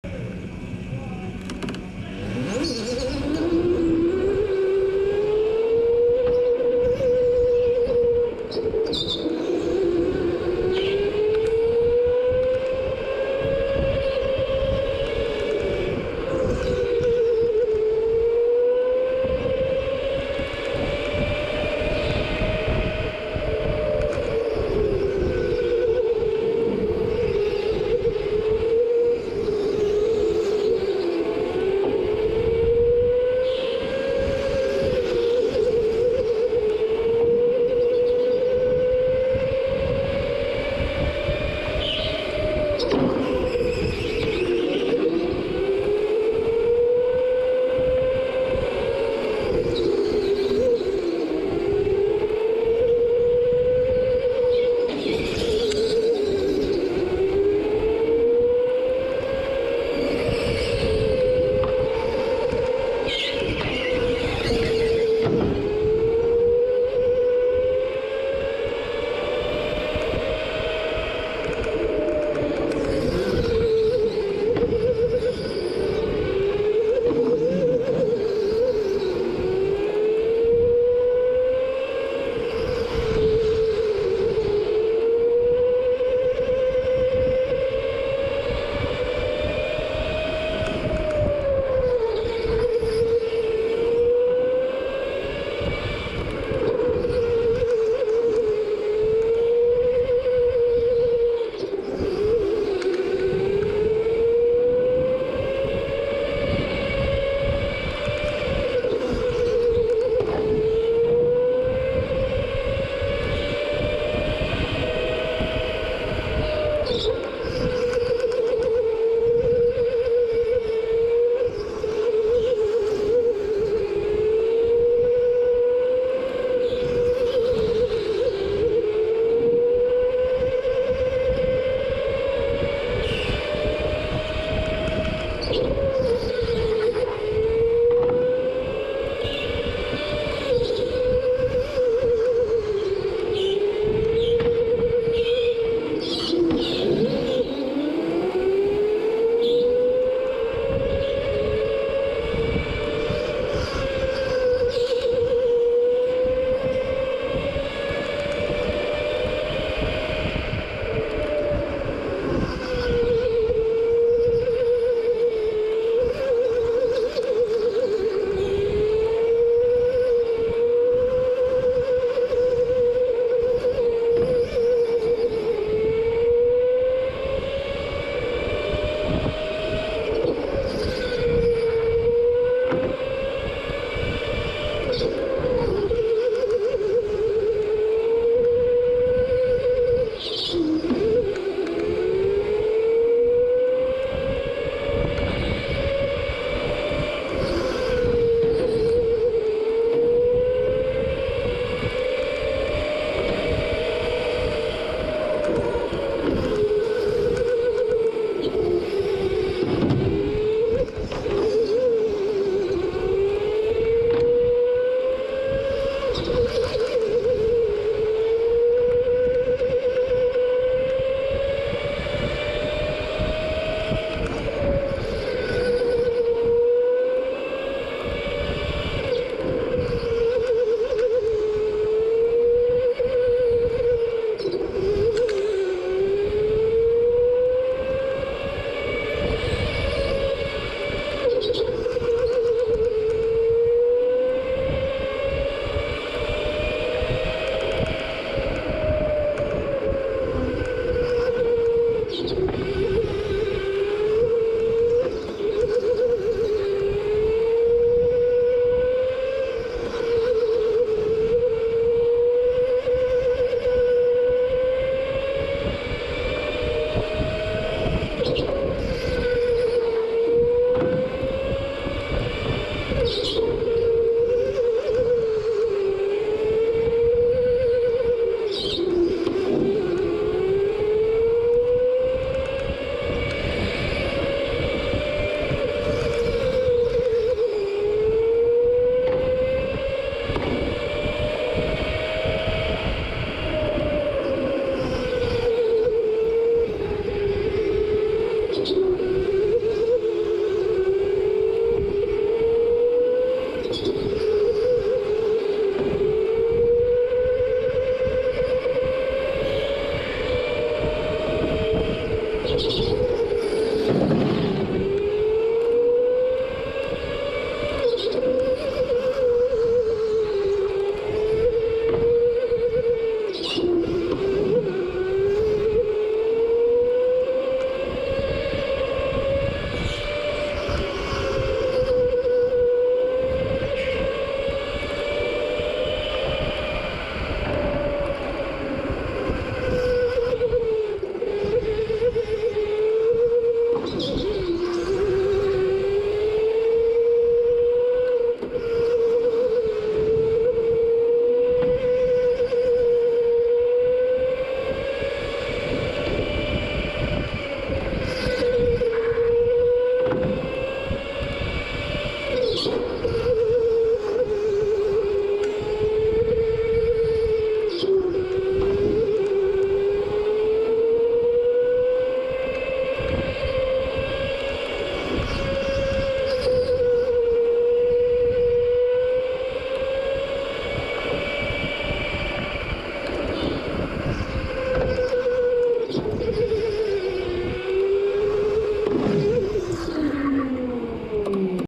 High Caliber Karting Mega Track Race 4k 60FPS POV
This is at High Caliber Karting in Okemos Michigan on their "Mega Track" that they open up a couple times a year.  I am a novice, and this was my third visit.